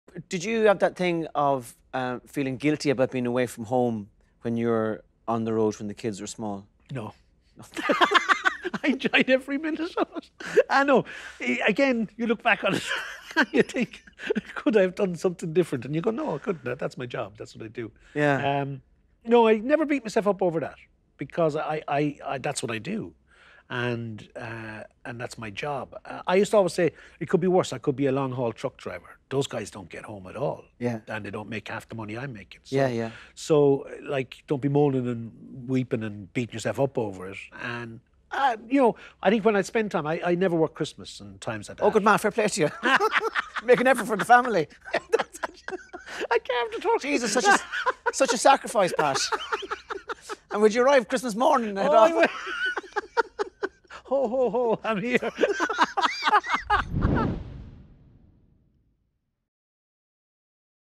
“That’s my job, that’s what I do.” | RTÉ
Pat Shortt has no regrets about being on the road when his family was young and was always home for the Christmas dinner 😉😉